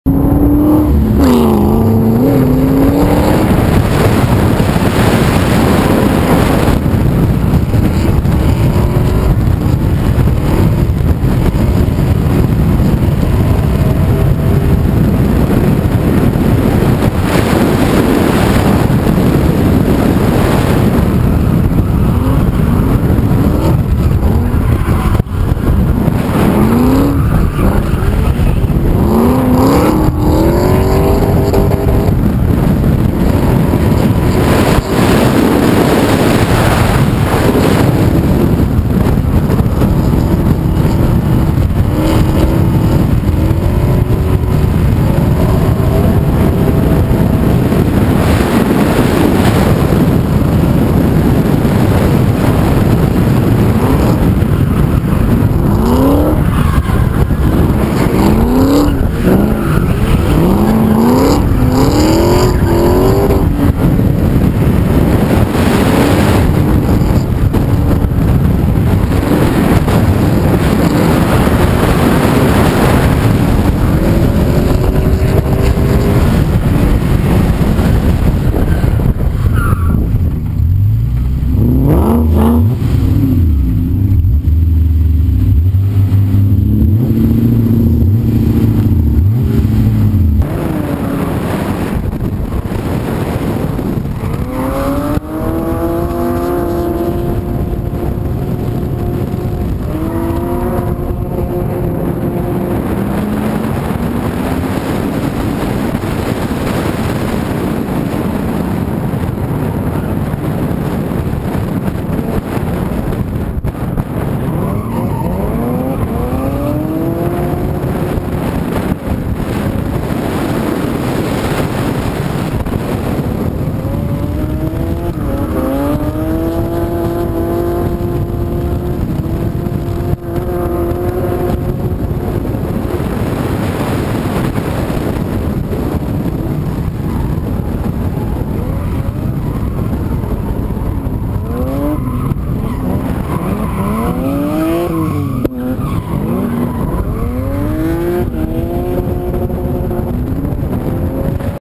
Driftland UK first ever test
Raw footage of the 1st ever drift of the first purpose built drift track in the UK. The guys from 7eam CHIZFAB were invited down for a super secret test night .